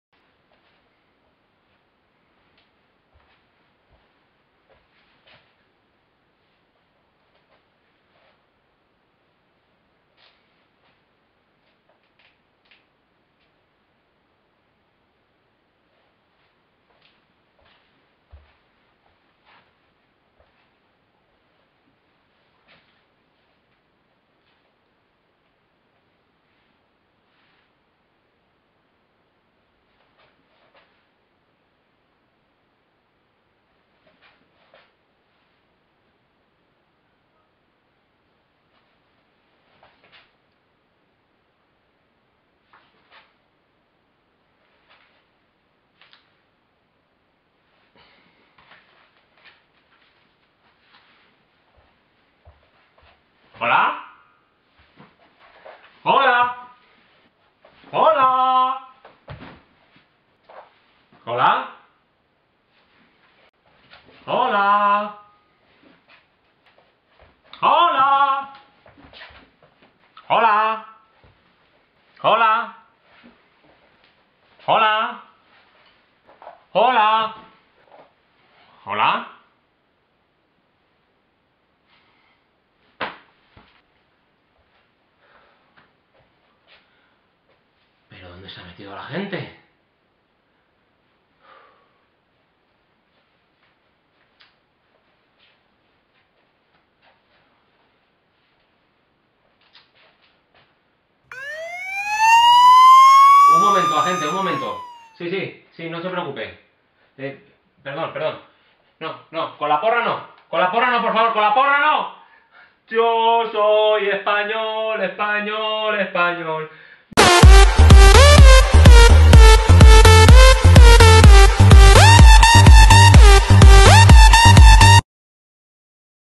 Mi viaje a Nueva York en pleno confinamiento. No te imaginarás como acaba!
En pleno confinamiento consigo un viaje muy barato a Nueva York. Nunca me imaginé que me podía pasar lo que muestro en el vídeo. A lo mejor no debería haber salido en plena cuarentena...